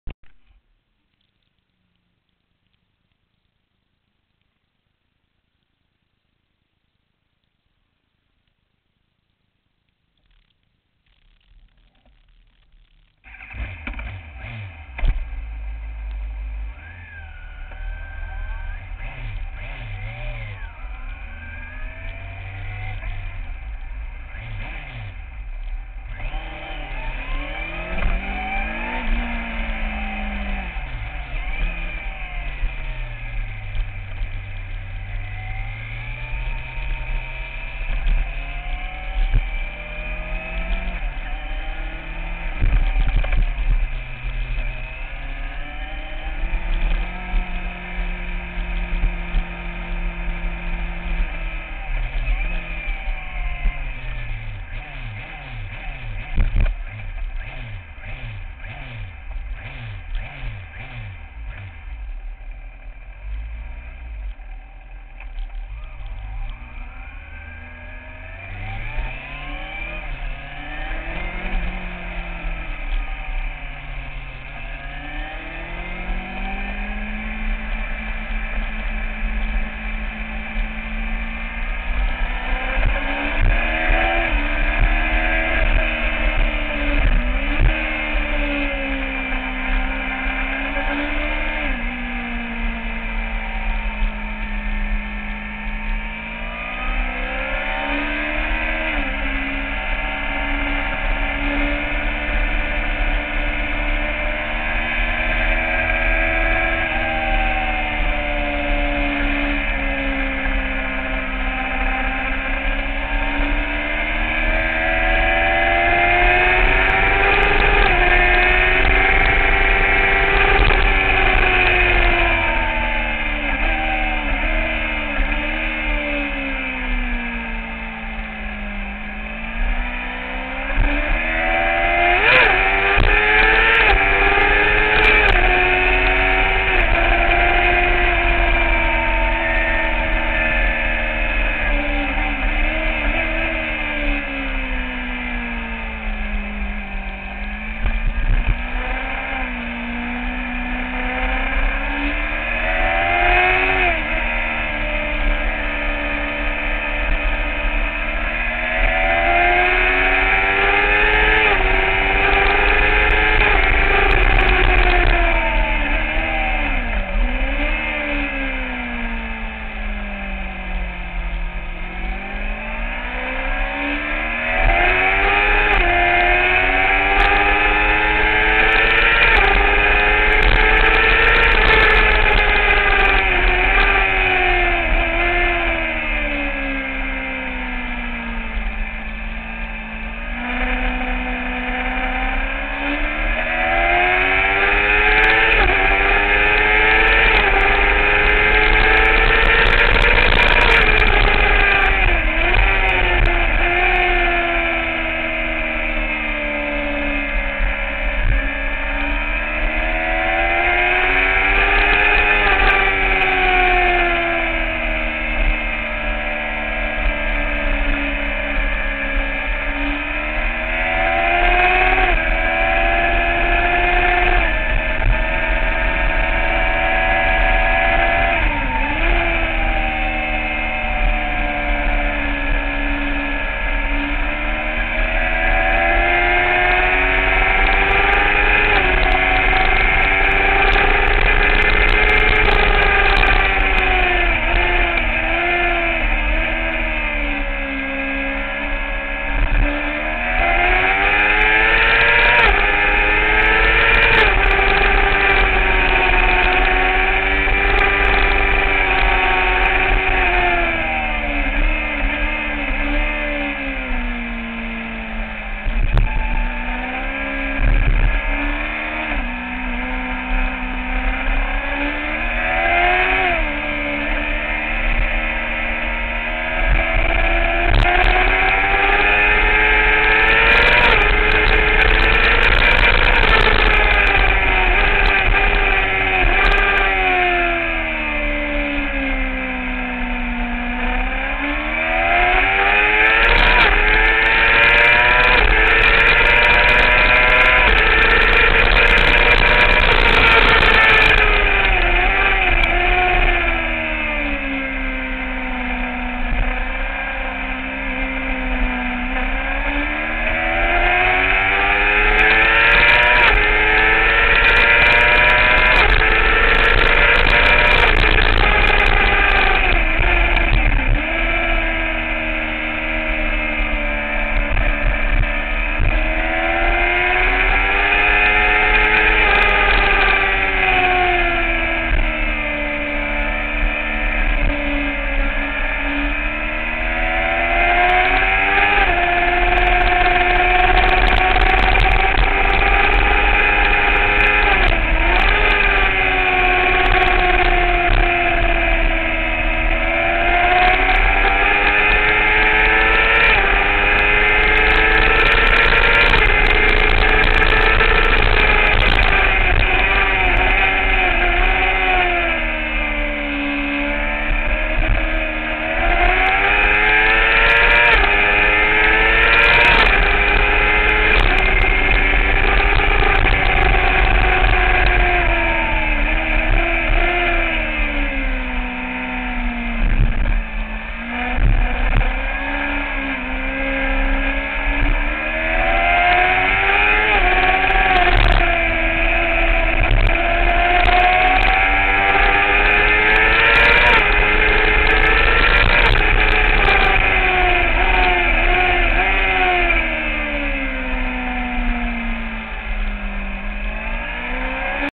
Starboy free practice @ Zolder II
Rider: Jan Van Steelandt

Bike: 09' Kawasaki Zx6r Monster Energy

Circuit: Zolder (Belgium)

Cam: GoPro Motorsport Hero (Available @ Wheelie World)